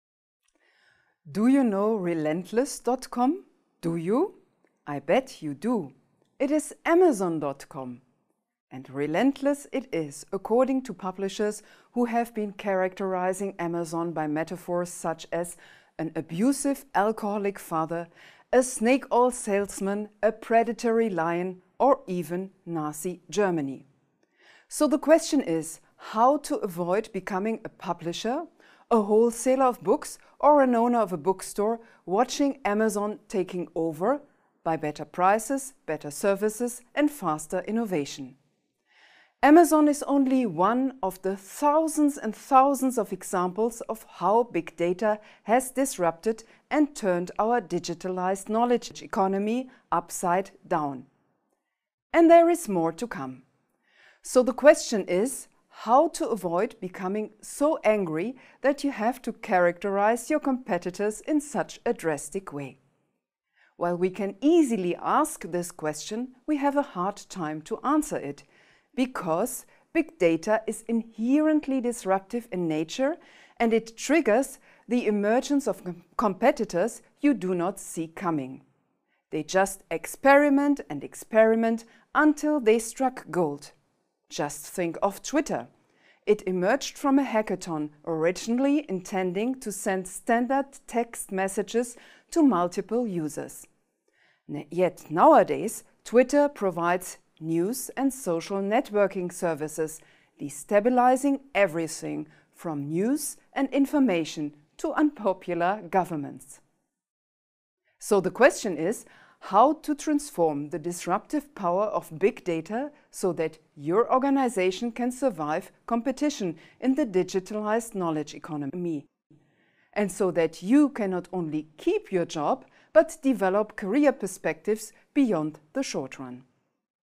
Big Data Strategies to Transform Your Business | Delft on edX
Make your organization’s business strategy and model, as well as your own career path, future-proof by using big data’s disruptive power.

While big data infiltrates all walks of life, most firms have not changed sufficiently to meet the challenges that come with it. In this course, you will learn how to develop a big data strategy, transform your business model and your organization.

This course will enable professionals to take their organization and their own career to the next level, regardless of their background and position.

Professionals will learn how to be in charge of big data instead of being subject to it. In particular, they will become familiar with tools to:
assess their current situation regarding potential big data-induced changes of a disruptive nature,
identify their options for successfully integrating big data in their strategy, business model and organization, or if not possible, how to exit quickly with as little loss as possible, and
strengthen their own position and that of their organization in our digitalized knowledge economy
The course will build on the concepts of product life cycles, the business model canvas, organizational theory and digitalized management jobs (such as Chief Digital Officer or Chief Informatics Officer) to help you find the best way to deal with and benefit from big data induced changes.

During the course, your most pressing questions will be answered in our feedback videos with the lecturer. In the assignments of the course, you will choose a sector and a stakeholder. For this, you will develop your own strategy and business model. This will help you identify the appropriate organizational structure and potential contributions and positions for yourself.